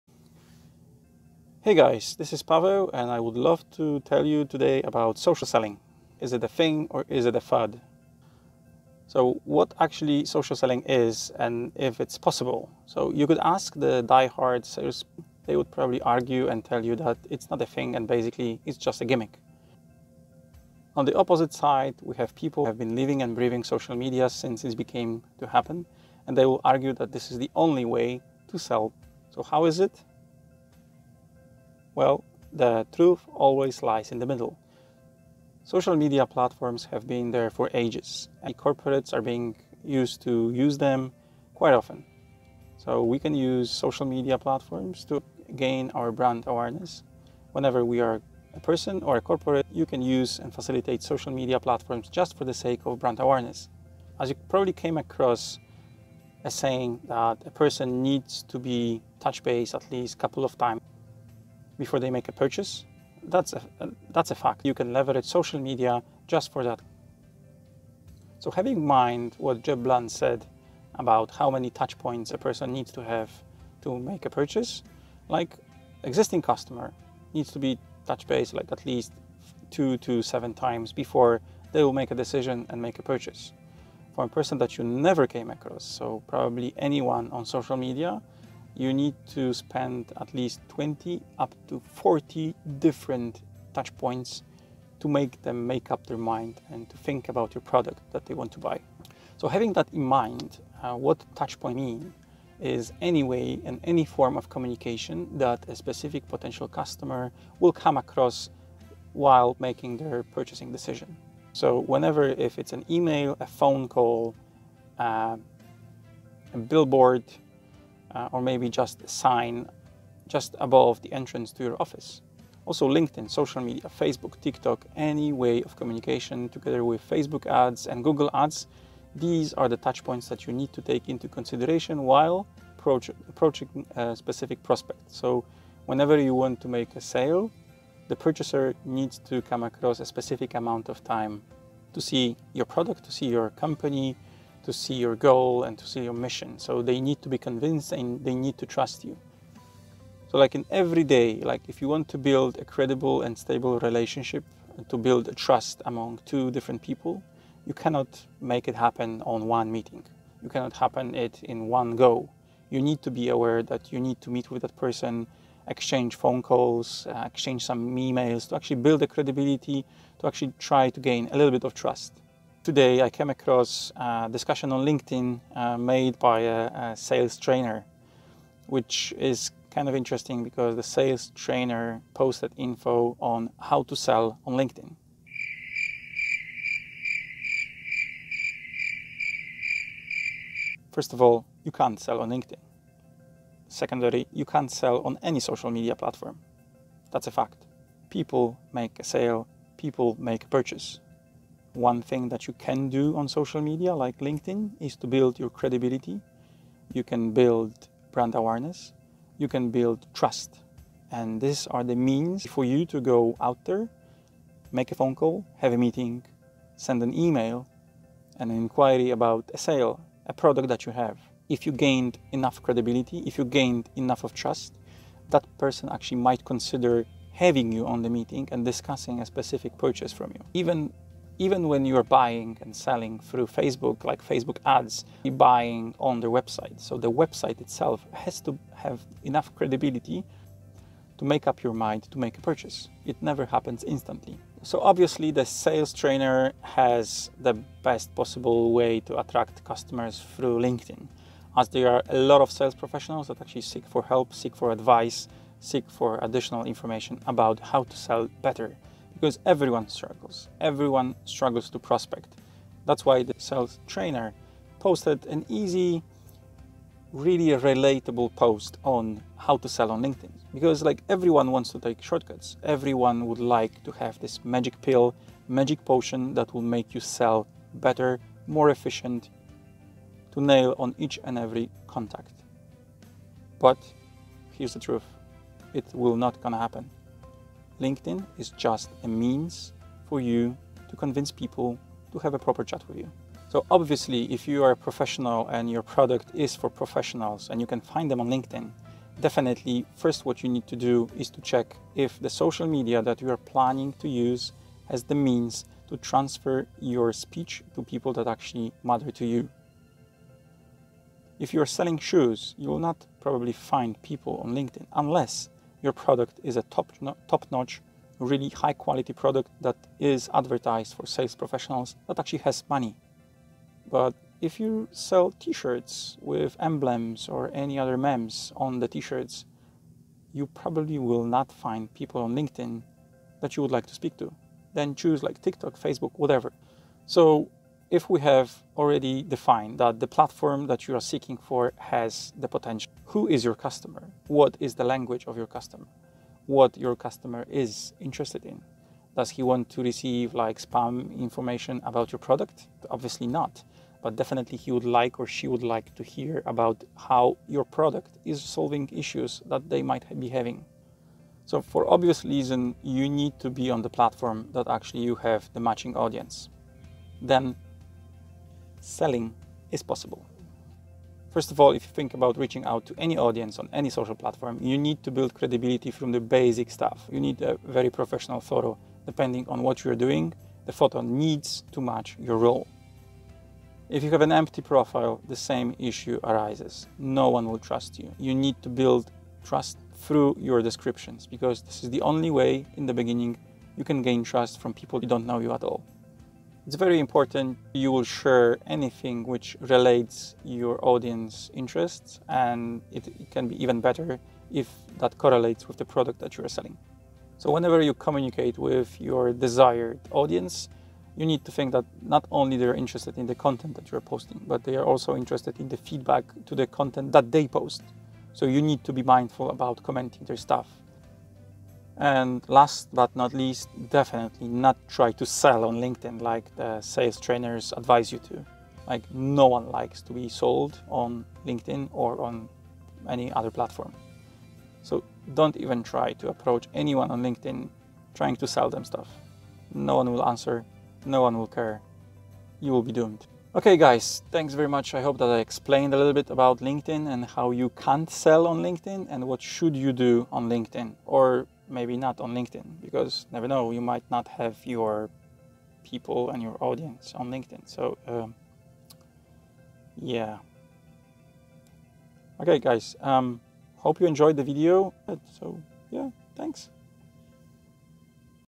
You can't sell on LinkedIn - The Myth of Linkedin Social selling 2019
Sales trainers are furious as we debunk social selling secrets.
Number one rule: YOU CAN'T SELL ON LINKEDIN